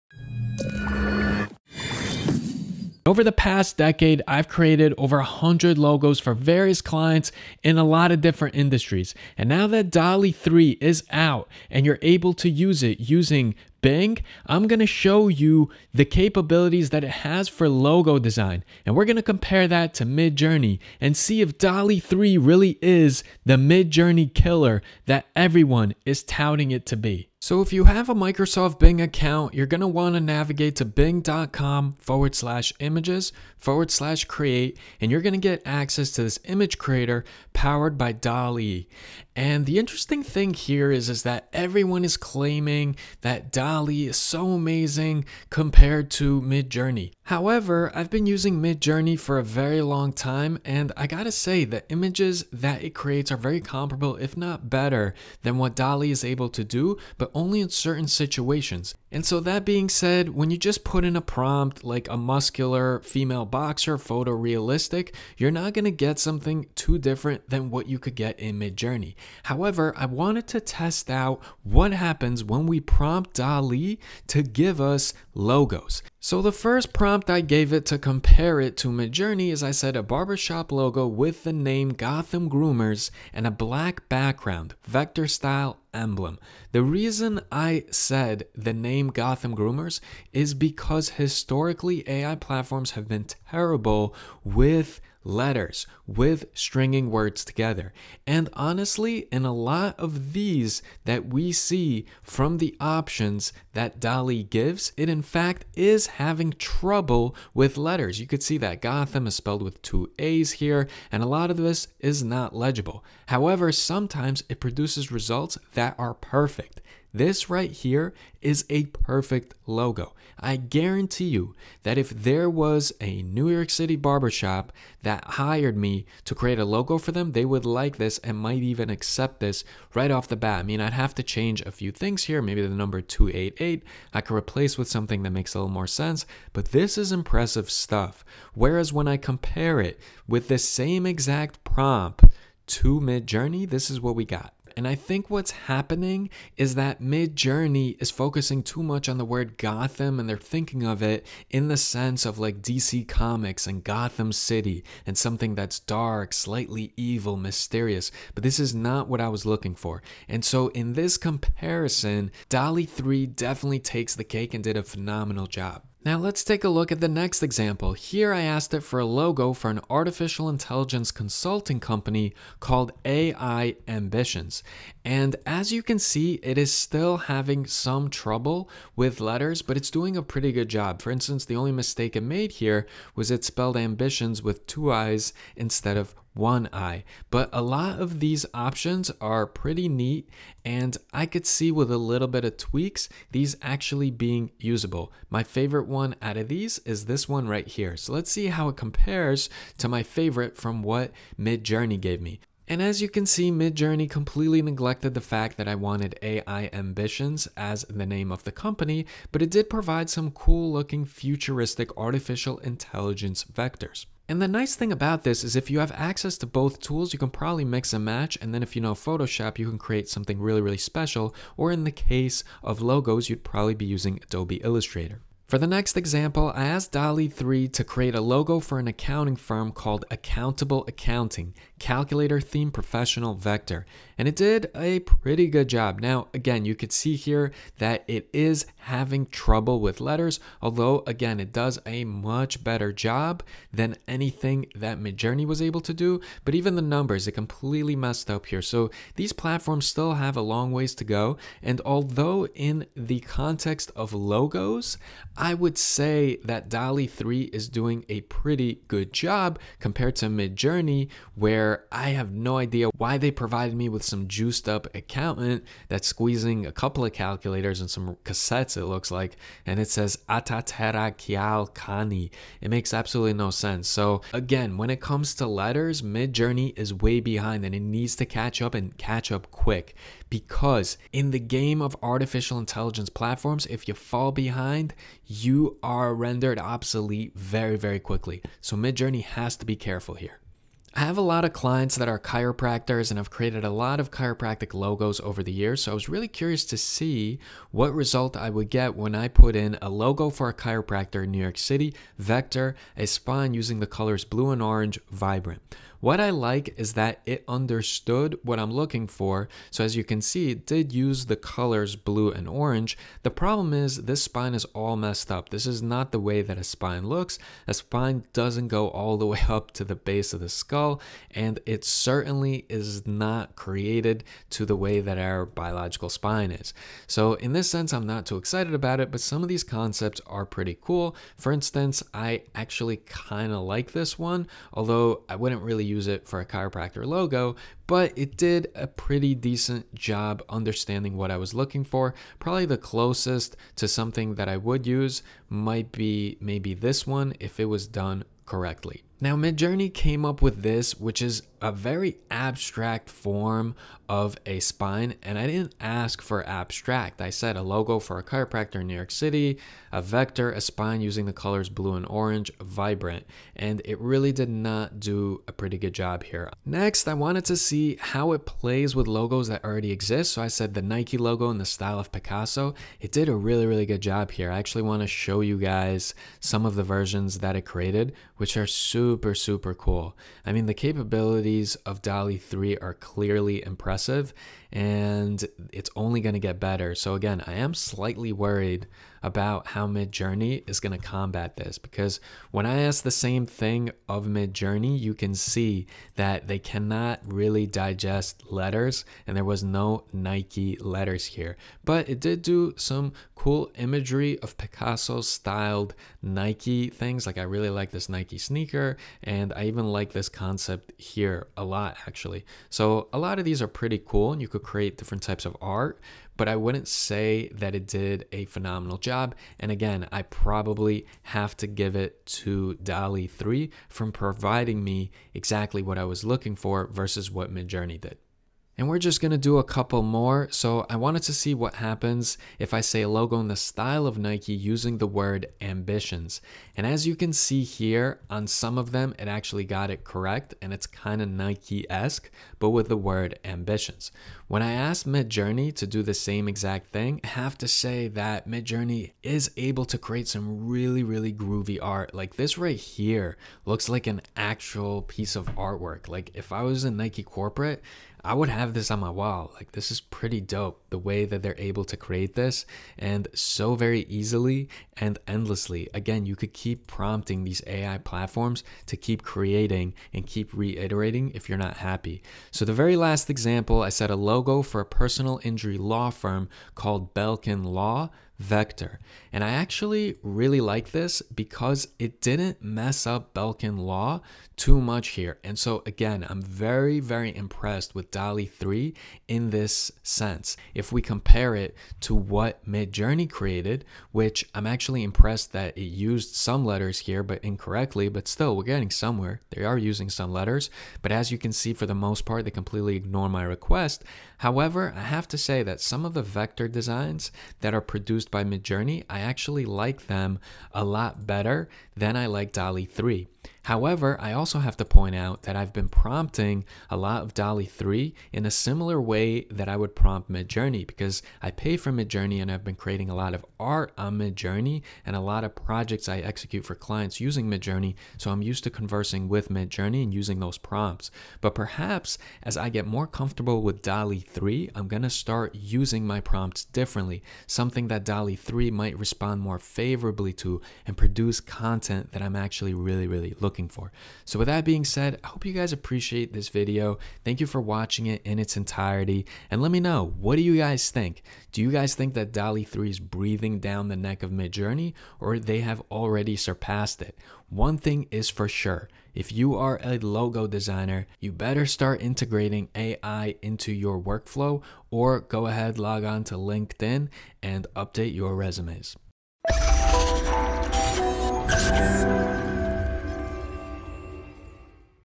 Dalle3 AI Logo Designs Are Impressive! Is Midjourney in trouble?
How to create logo designs using Open AI Dalle 3 platform is detailed in this video and then we do a comparison of Dalle 3 capabilities vs that of Midjourney.

Let's talk about it in the comments section...